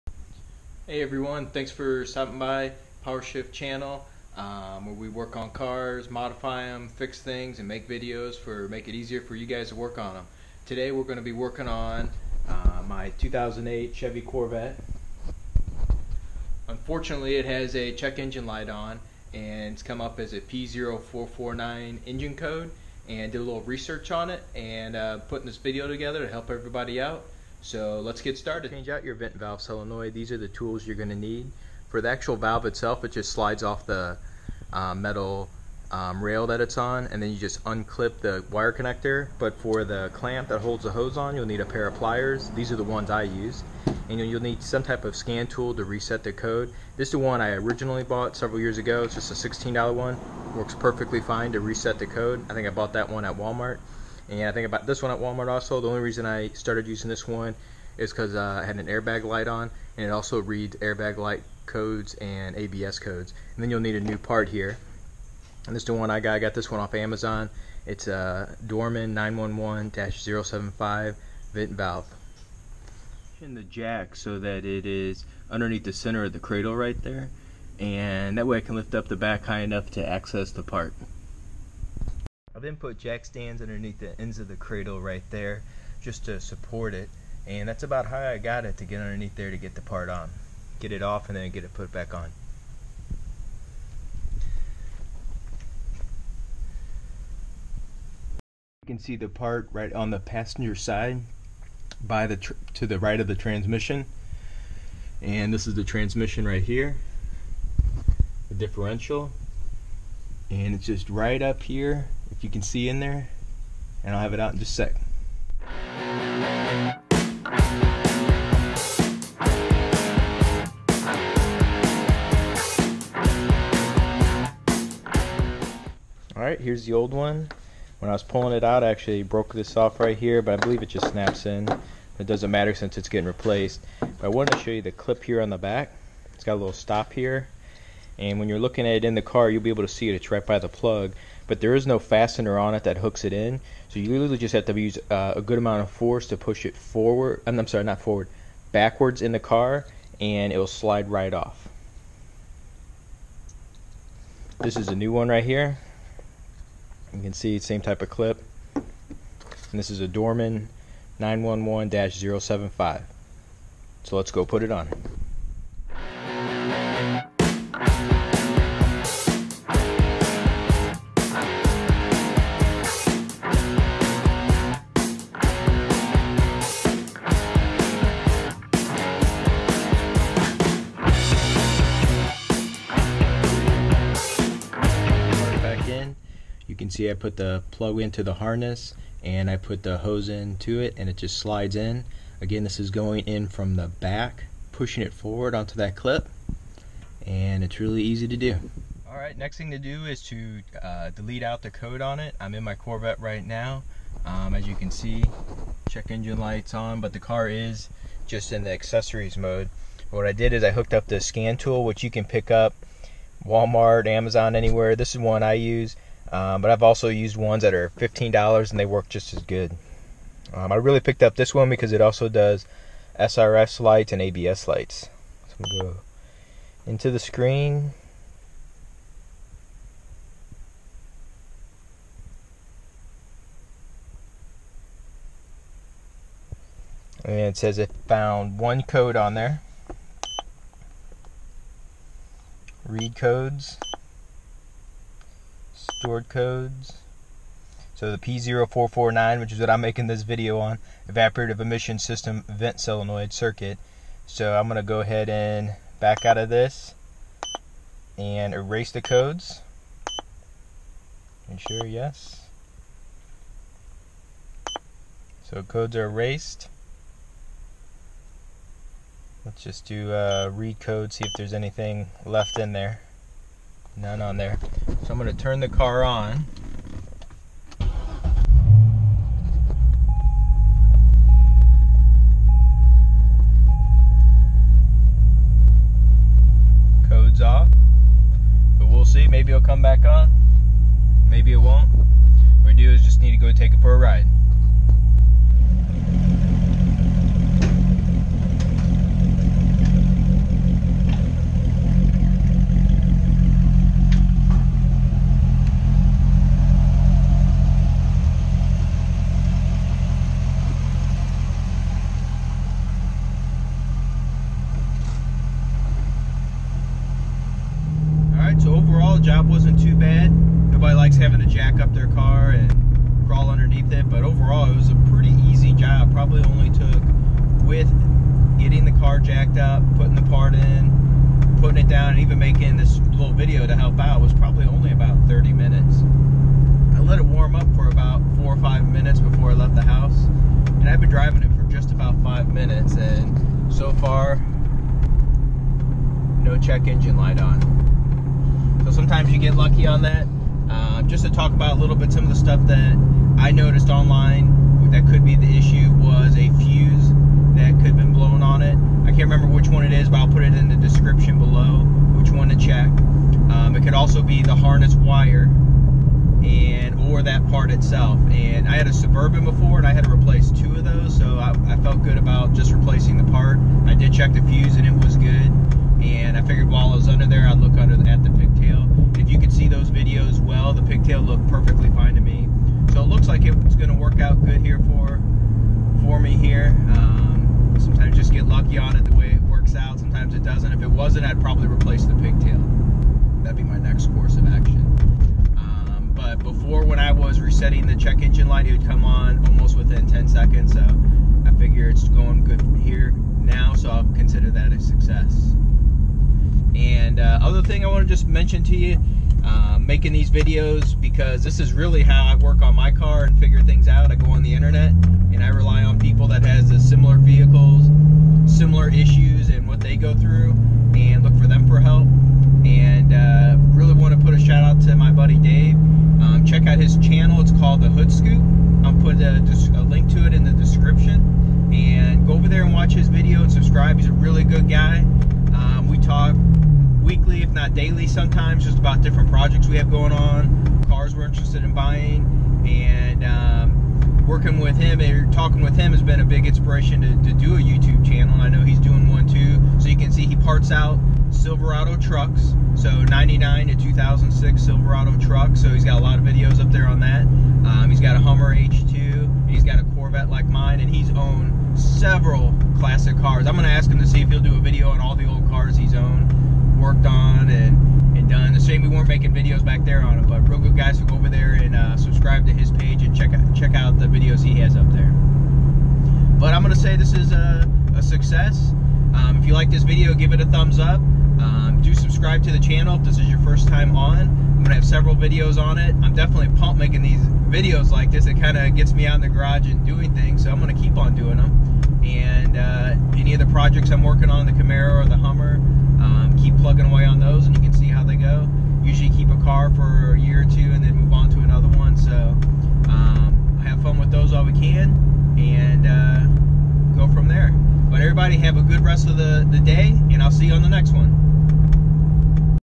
PO449 Check Engine Light Replacing Evaporative Vent Valve Solenoid on Corvette C6 Dorman 911-075
Replaced the evaporative vent valve solenoid on my corvette c6 because I had a P0449 check engine light on.

Check the following to determine what is wrong
1. Check 25 amp Fuse #13 under the hood.
2. Check pig tail to evaporative vent valve solenoid
3. Possible replace evaporative vent valve solenoid

Replacing part# Dorman 911-075 Vapor Canister Vent Solenoid.